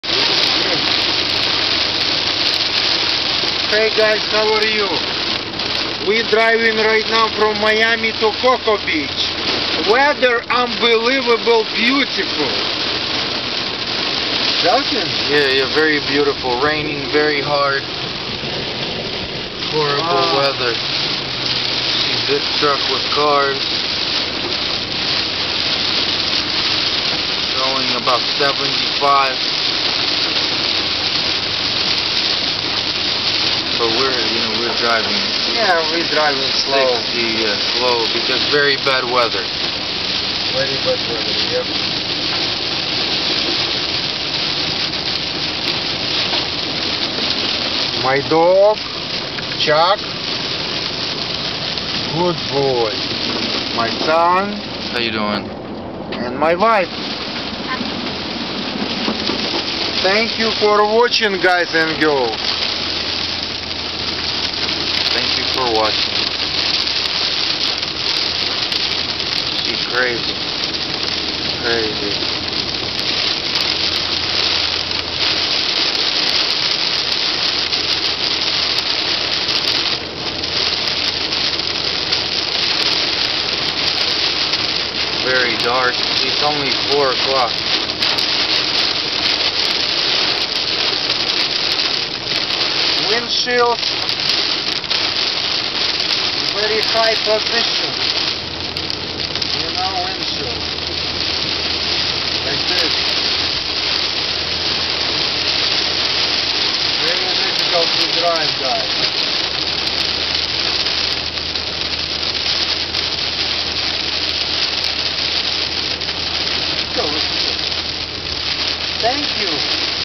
STORM Driving to MIAMI from COCOA BEACH FL FLORIDA USA
STORM Driving to MIAMI from COCOA BEACH FL FLORIDA USA - Family Vacation MAY 2016